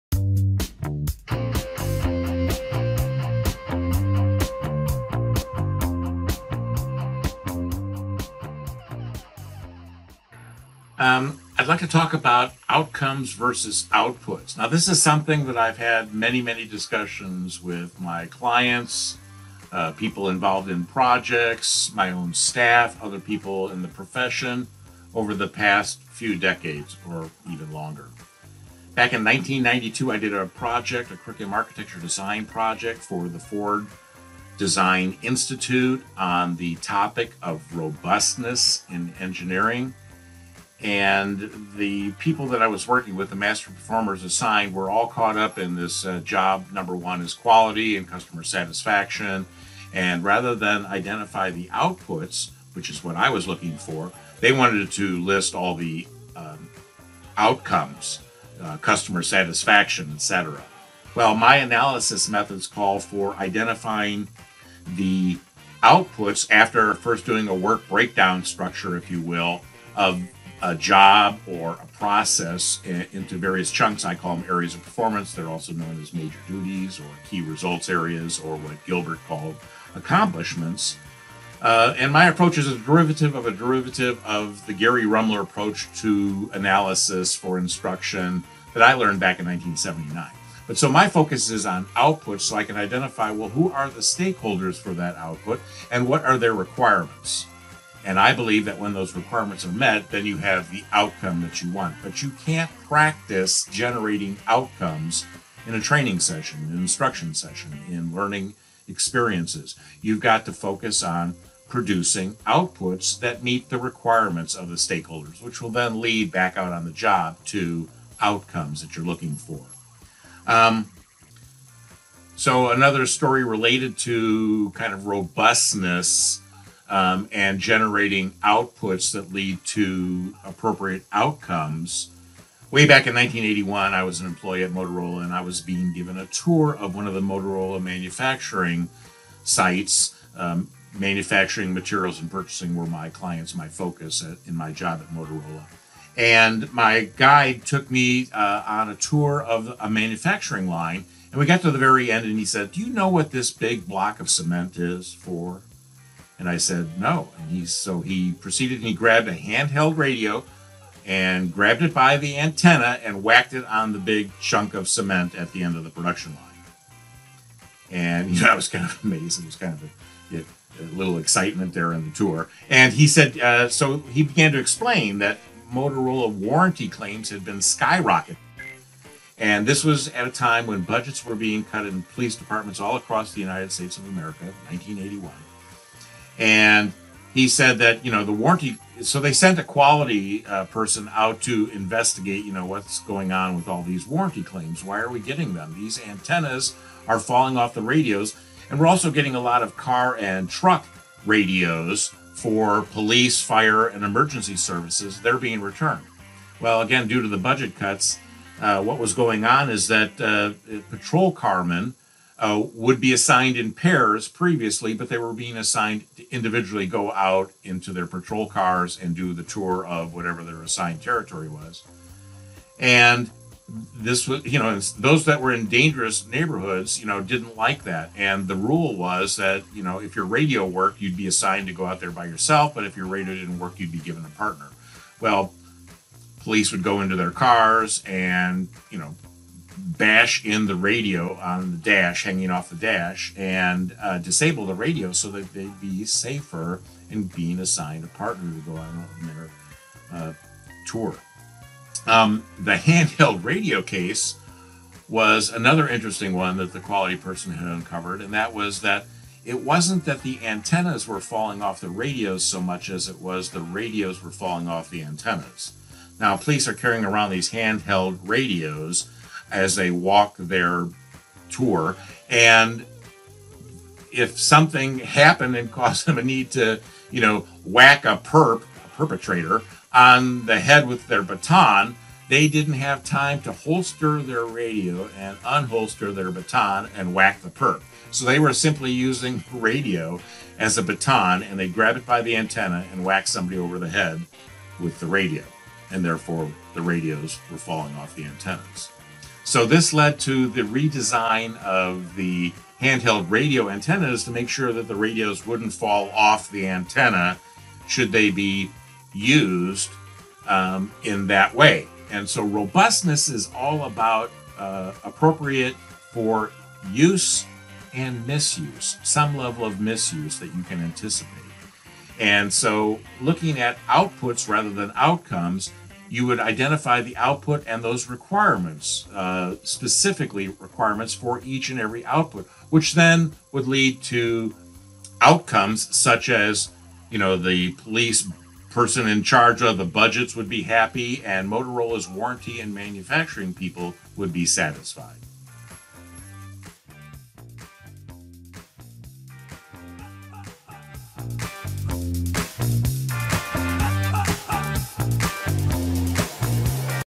GFP 2022 - Outcomes vs Outputs
Go For Performance 2022 - Outcomes vs Outputs - I’ve had quite a few discussions with people on this over the past few decades – or more. Here are two related stories about Outcomes vs Outputs.

License